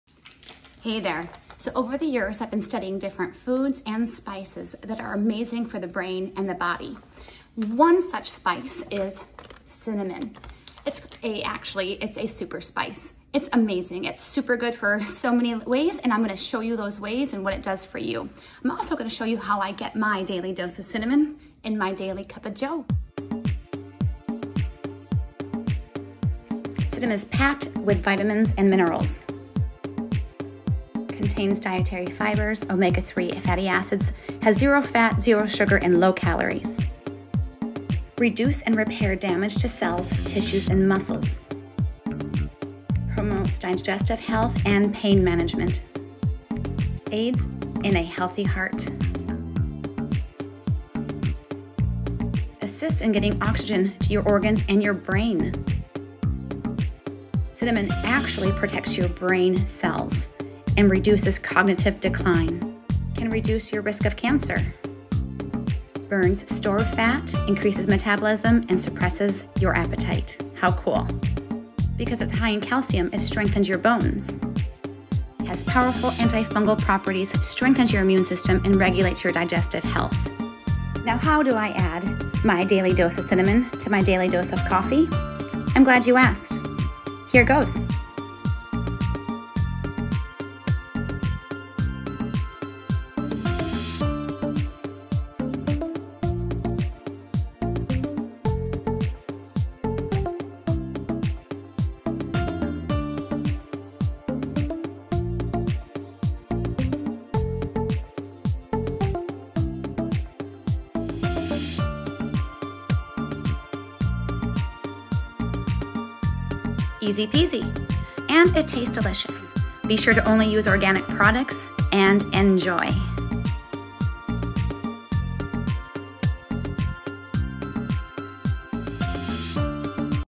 Organic Cinnamon Jo -your daily does of body and brain goodness
Recommendation is to "ease" into your daily dose of Cinnamon.  Recommended Daily Dose is 1 tablespoon.  Start with a quarter tablespoon and add gradually.  Cinnamon is full of vitamins and minerals... when I first started adding cinnamon, I had a slight nausea feeling - this happens when I take vitamins.  I learned that a gradual introduction is the best way to get my now full dose of Cinnamon everyday!